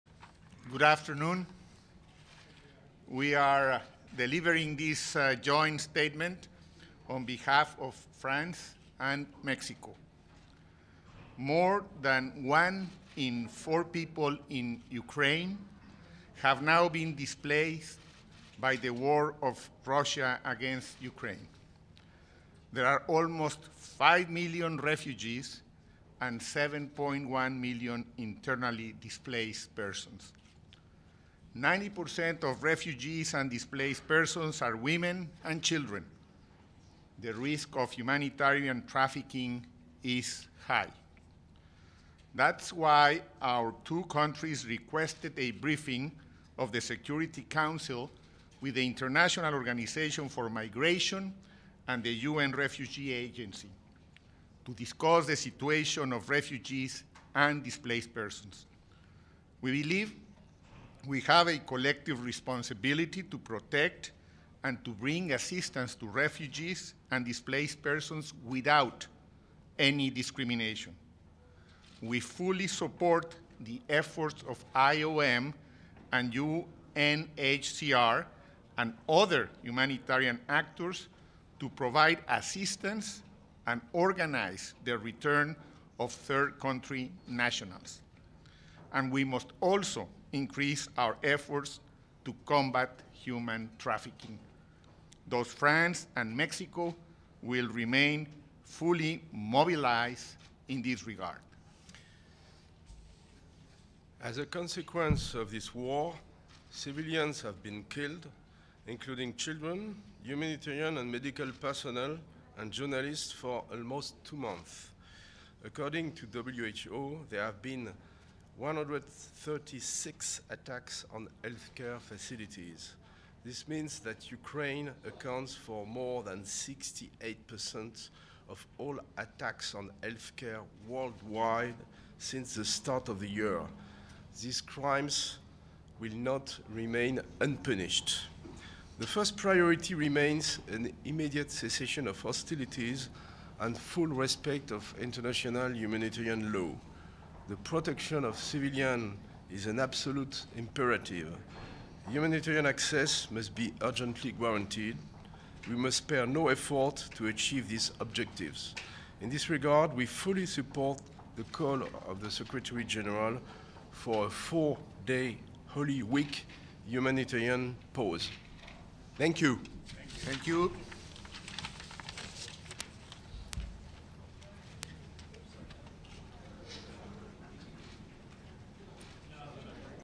France and Mexico on Ukraine - Security Council Media Stakeout (19 April 2022)
Joint stakeout by Nicolas de Rivière, Permanent Representative of France to the United Nations, and Juan Ramón de la Fuente, Permanent Representative of Mexico to the United Nations, on the humanitarian situation in Ukraine.